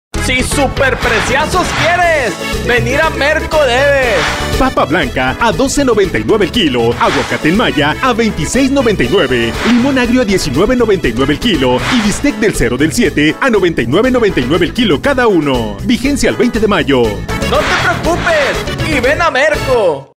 18-20 Mayo-2021 | Nuevo León
Si buscas Calidad, Frescura y Precio. No te Preocupes. Ven a Merco.
Ofertas válidas en Merco Zona Nuevo León
Martes 18, Miércoles 19 y Jueves 20 de Mayo, 2021.